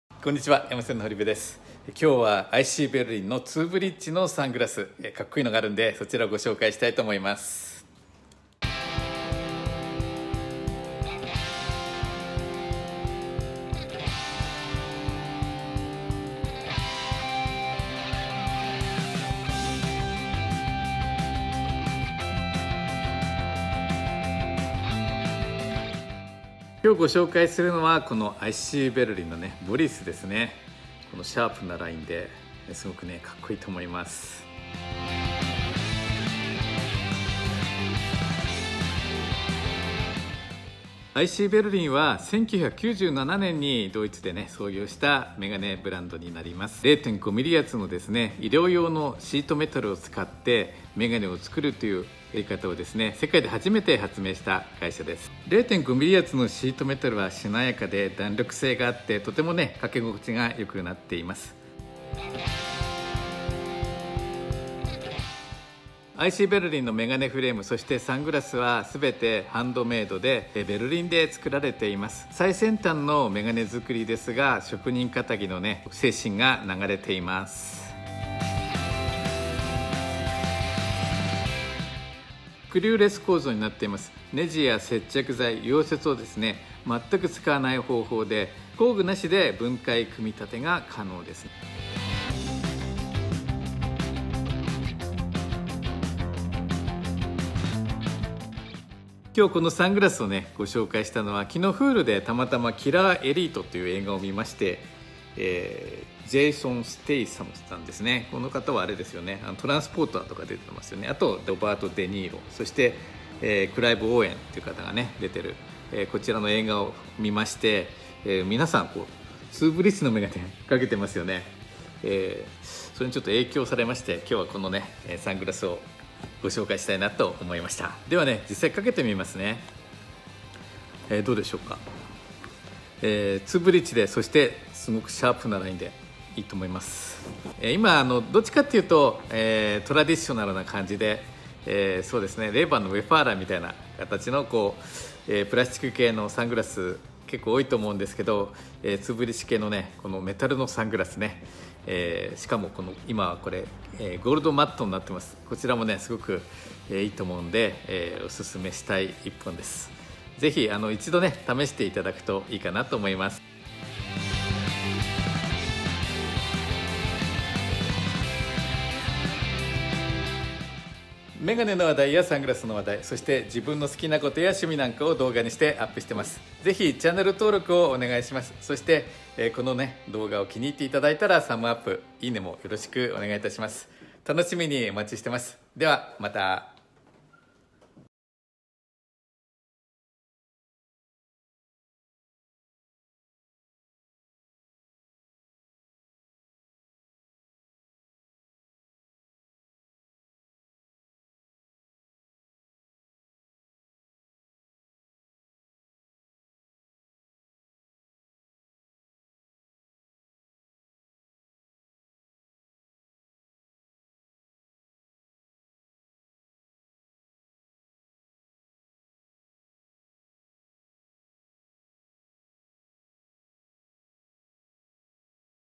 【ic! berlin】キラー・エリート見てたら紹介したくなりました! ツーブリッジサングラス好きです!! boris n. （Premiere Rush CCで編集） 2018年10月30日
Huleでキラー・エリートという映画を見ていたら紹介したくなりました。かっこいいツーブリッジ・サングラスです!!

〒227-0061 横浜市青葉区桜台29-16

使用機材
iPhone X
Macbook Pro
Hovercamera

使用アプリ
Abobe Premiere Rush CC（iOS)
Abobe Premiere Rush CC（Mac OS）